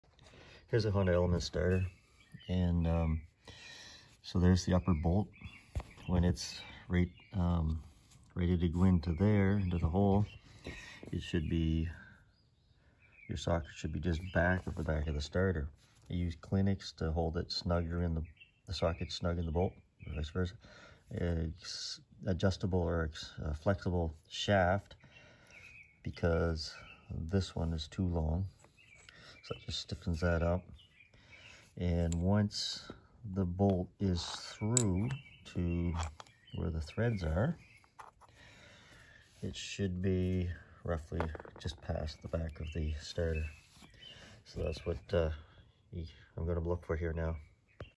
Upper starter bolt alignment and tools used Honda Element
Honda element 2010 starter upper bolt alignment and tools used.  Use bottom bolt hole as reference to upper hole and distances of upper bolt and socket to entry of starter hole and threaded hole on block.
Compare to the removed starter for reference.